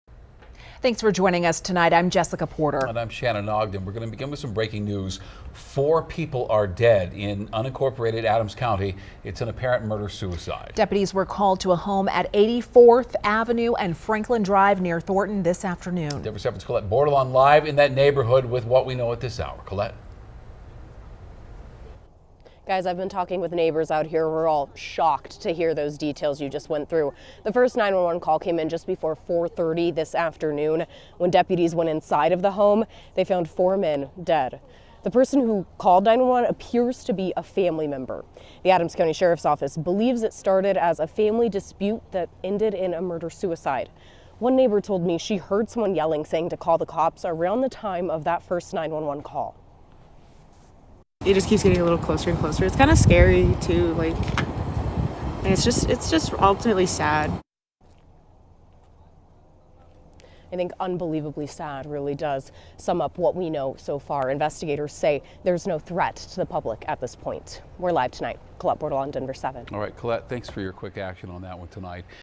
Four men found shot to death in Adams County home late Tuesday afternoon in apparent murder-suicide
Deputies are investigating after four men were found shot to death in an apparent murder-suicide inside an Adams County home late Tuesday afternoon.